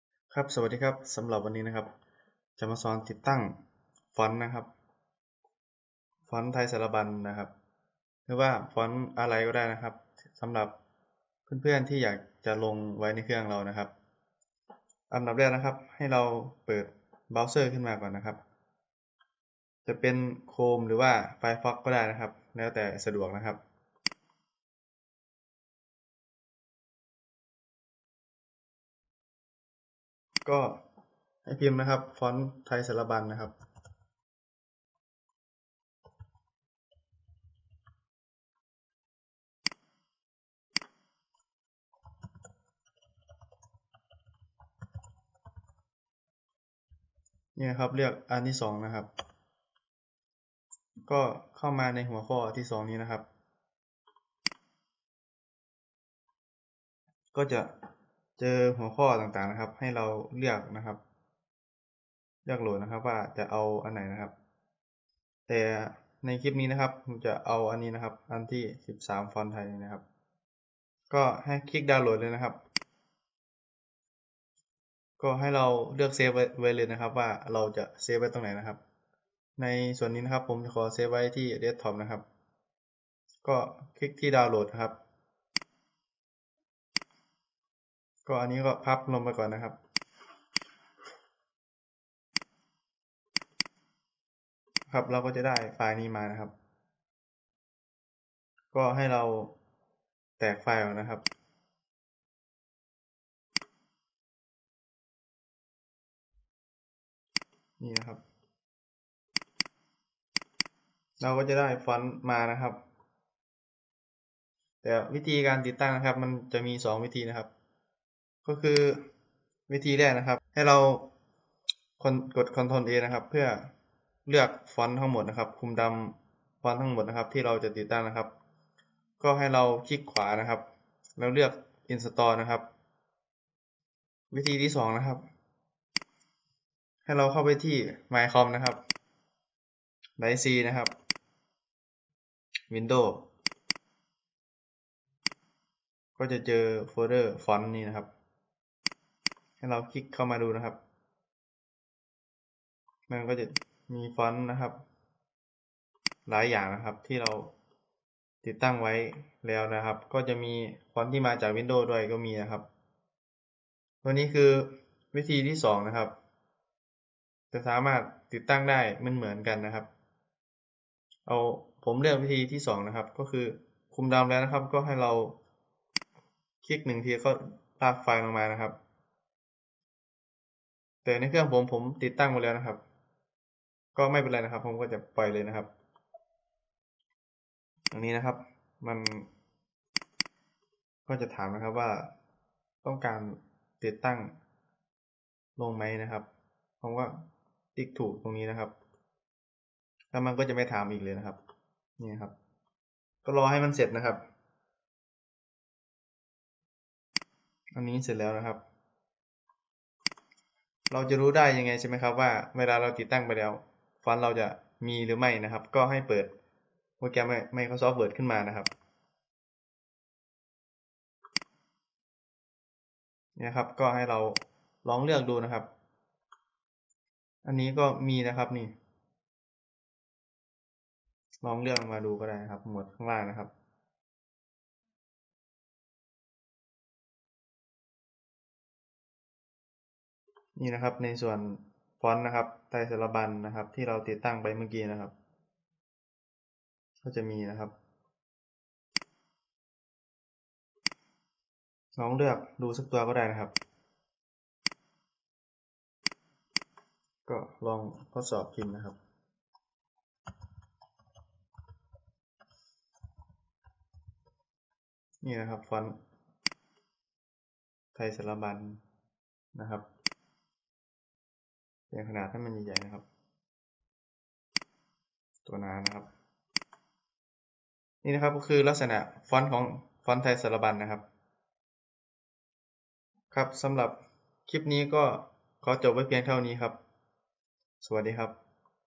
ติดตั้งฟ้อนไทยสารบัญ
คลิปนี้ลองแบบไม่เห็นเมาส์ดูจะเป็นยังไง
ดาวน์โหลดฟ้อน